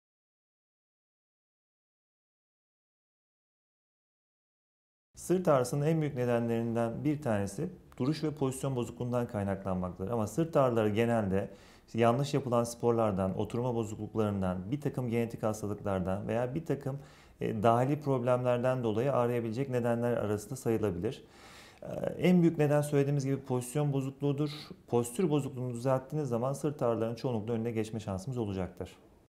Sırt neden ağrır?
Acıbadem Maslak Hastanesi, Ortopedi ve Travmatoloji Uzmanı Dr. Metin Uzun, sırt ağrısı, omuz çıkığı ve ayak sağlığı üzerine soruları yanıtlıyor;